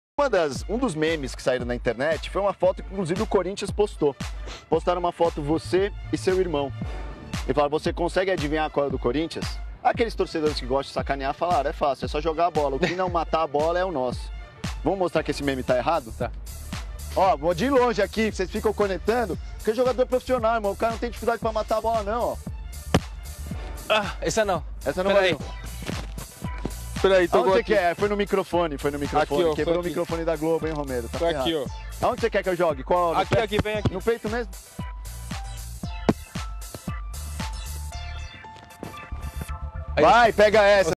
ROMERO SE COMPLICA POR CAUSA DO MICROFONE
ROMERO NÃO CONSEGUE MATAR A BOLA NO PEITO, POR CONTA DE MICROFONE DA GLOBO.